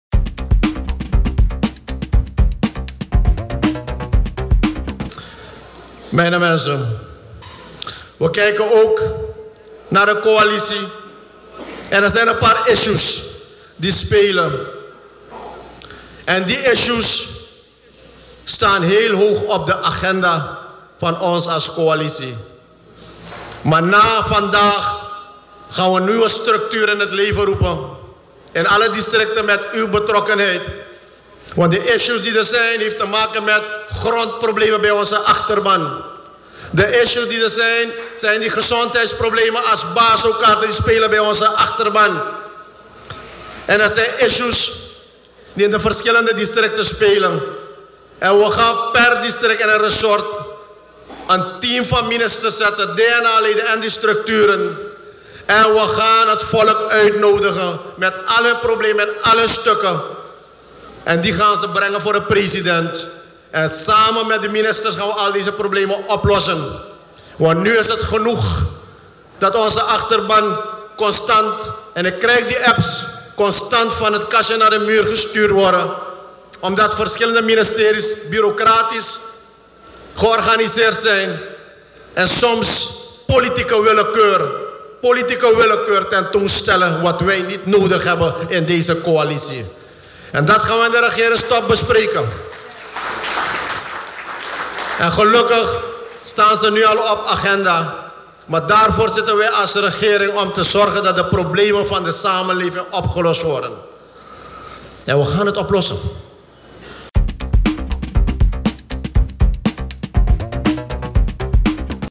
Aanpak tegenwerking VHP achterban
President tevens voorzitter van de VHP heeft meegedeeeld dat de achterban van de partij van het kastje naar de muur gestuurd wordt door onder andere “politieke willekeur” op bepaalde ministeries. Hij gaat dit aanpakken.